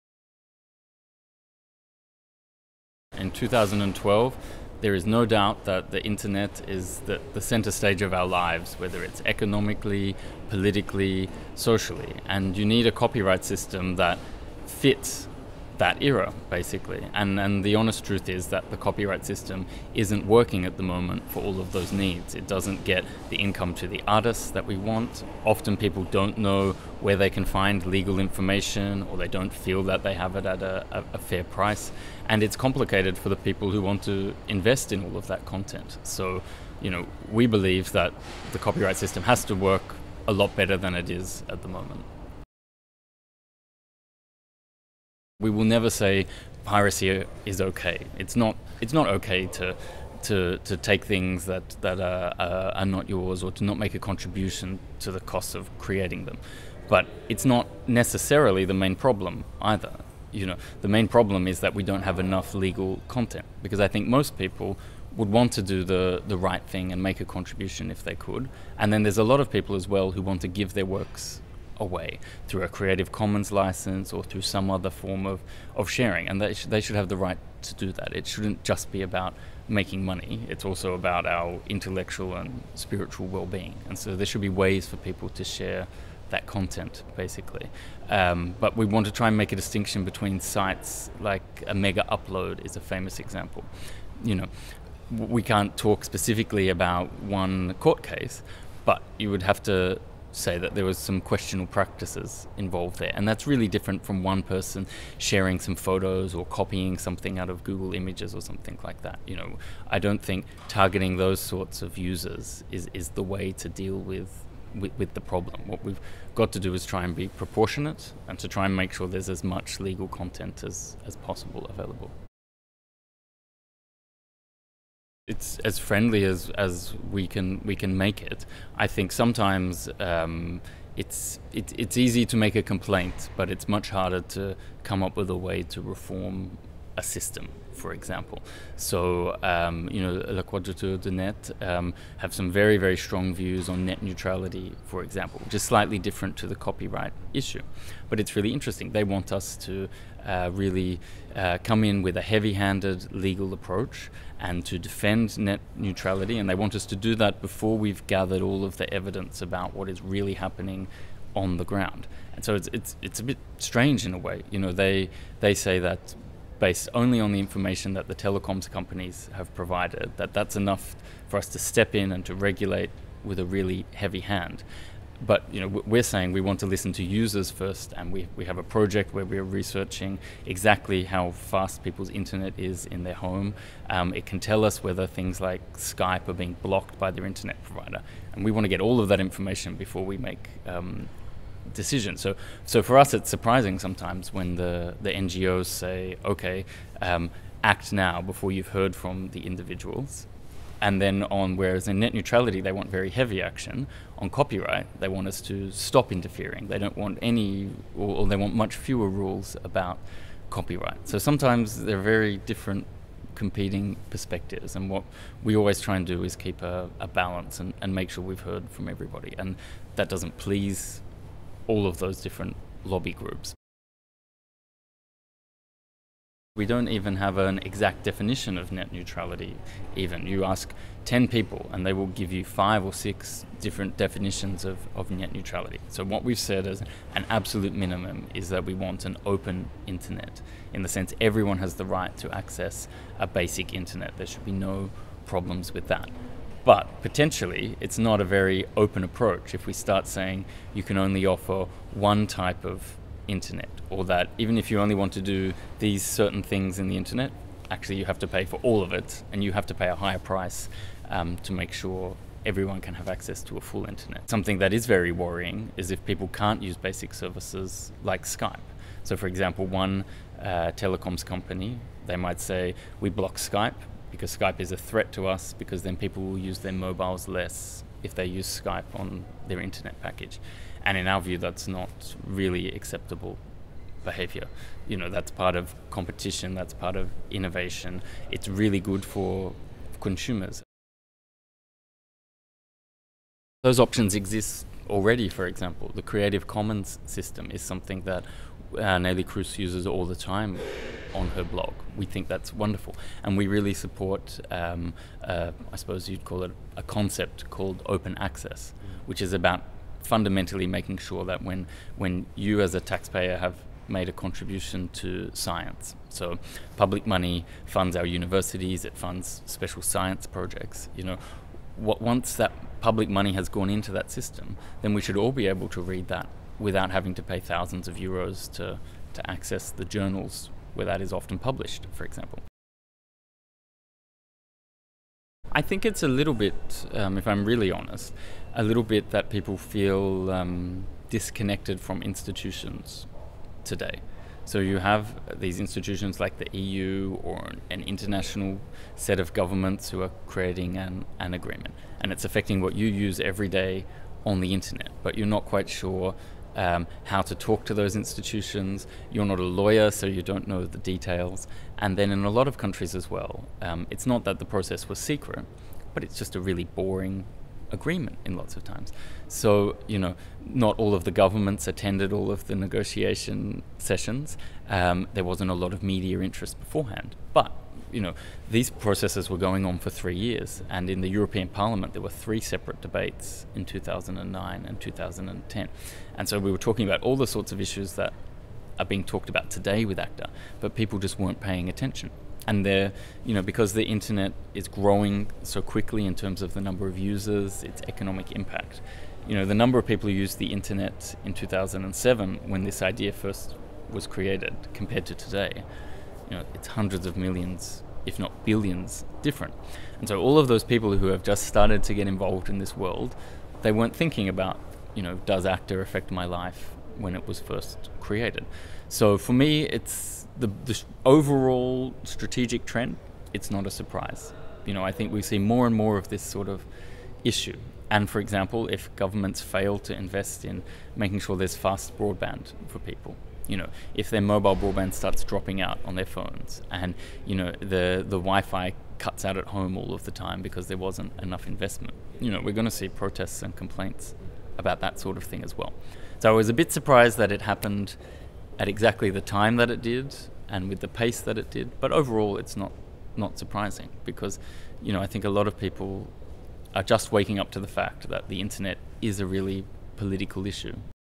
GKP / Polémiques : Le point de vue de la Commission européenne
En juillet 2012, la Commission européenne et plus particulièrement Karel De Gucht, le Commissaire au commerce, subissait une sérieuse déconfiture avec le rejet de l'Accord commercial anti-contrefaçon (ACTA) par un Parlement européen vexé par l'opacité ayant entouré la rédaction du texte. Trois mois auparavant, nous rencontrions Ryan Heath, porte-parole de Neelie Kroes, Commissaire à l'Agenda Digital, qui reconnaissait la nécessité de repenser le droit d'auteur mais pas celle de légiférer rapidement sur la neutralité du net.